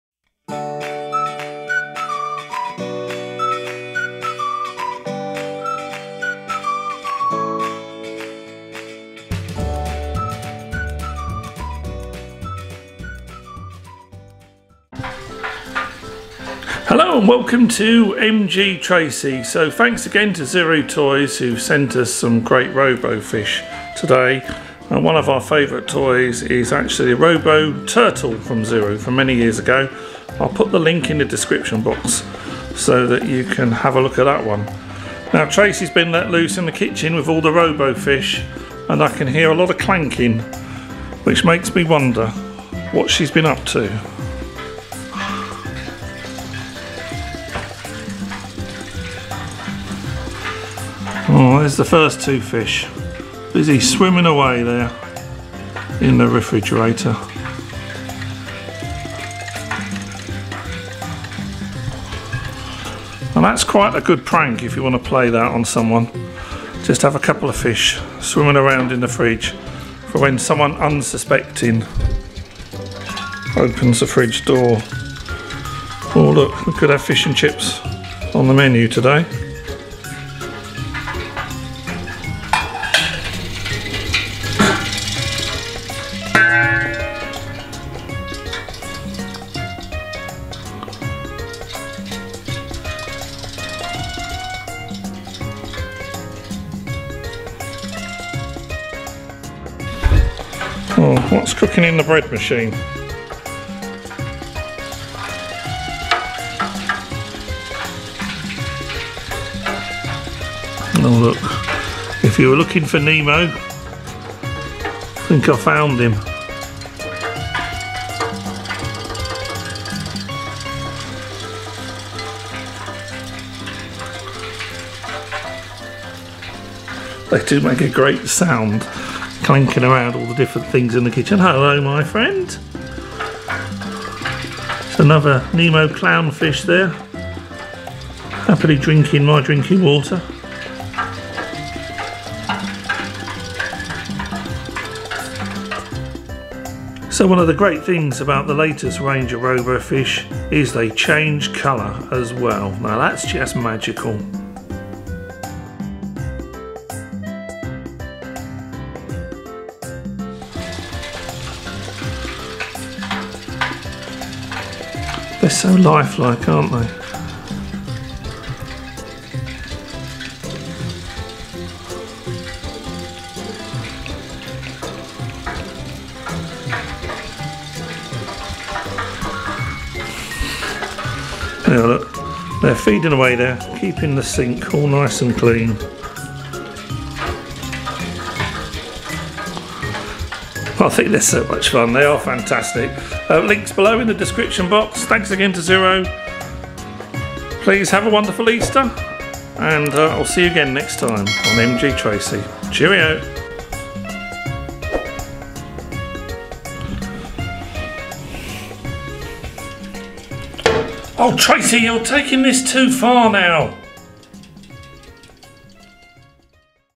Zuru Robo Fish 🐠, Robotic Fish 🐟 Swimming in my Kitchen
Here we have the latest colour changing Robo Fish by Zuru. What caught my eye was that this Robotic Fish looks like a Clown Fish and as I thought 'Finding Nemo' was a great movie thought it would be fun to have my very own digital electronic fish. without needing to feed or clean or look after a real fish. robofish pranks I think this is a very realistic looking fish and unless you look very closely - this looks just like a real Clown Fish Swimming in some odd places!
Zuru Robo Fish do have an 'Automatic turn off' timer where after 4 minutes the Robo Fish will stop swimming and float to the surface to save the batteries when left unattended.
🚒 Fireman Sam Delux Playset
🚒 Fireman Sam figures
Take care and stay safe - Tracey